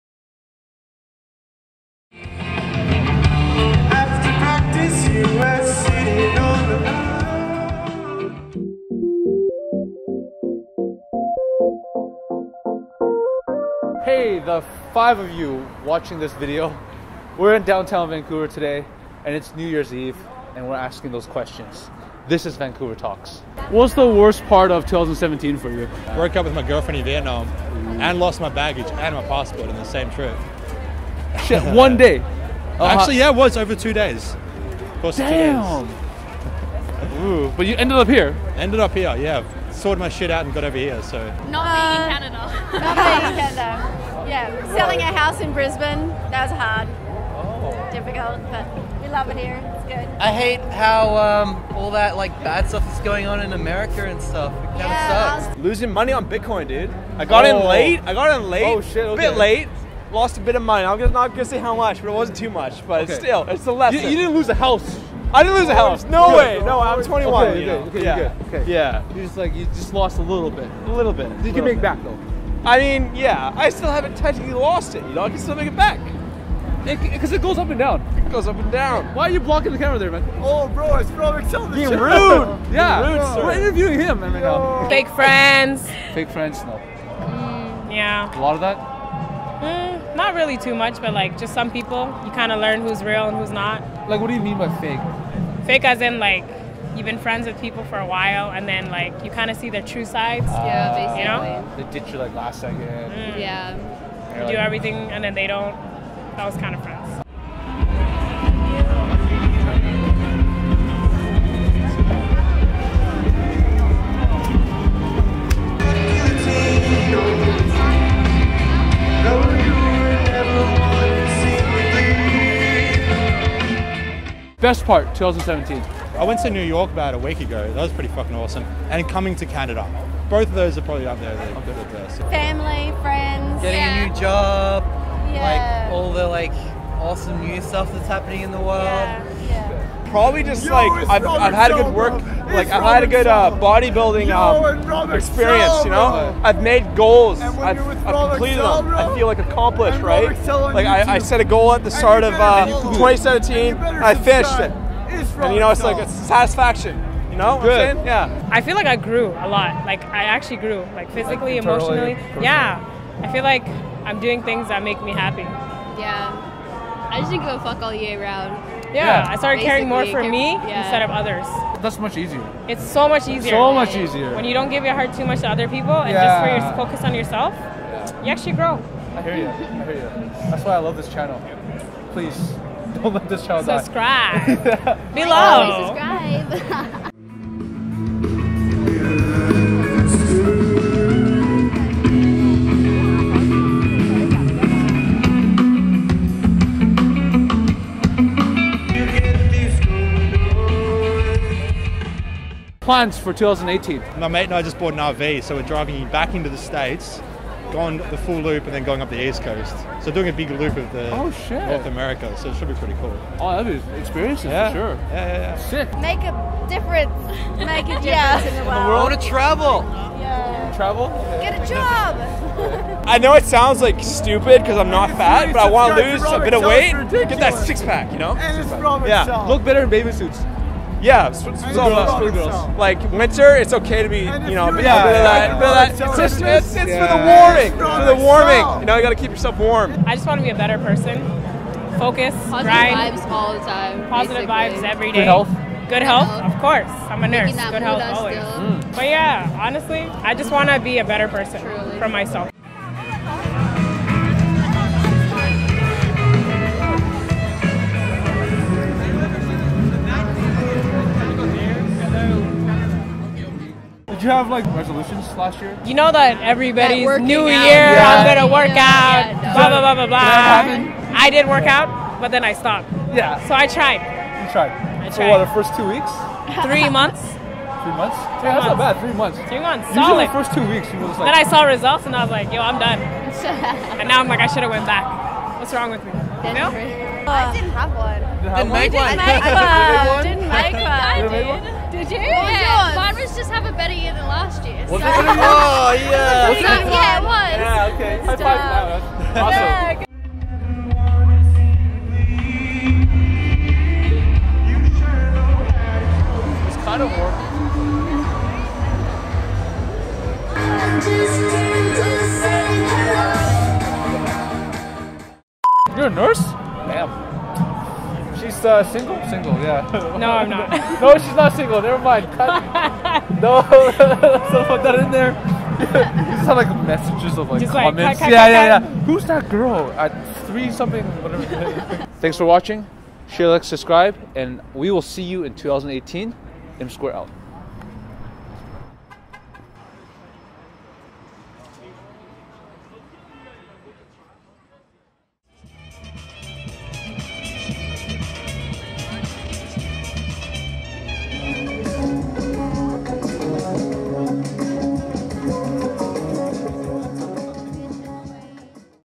New Years Eve 2017 - Vancouver Talks
New Years Eve 2017 & Happy New Year 2018!
Yup you guessed it, we asking questions!

[Shoutouts]
Hosts: Joe
Camera: Mario
Edit: Joe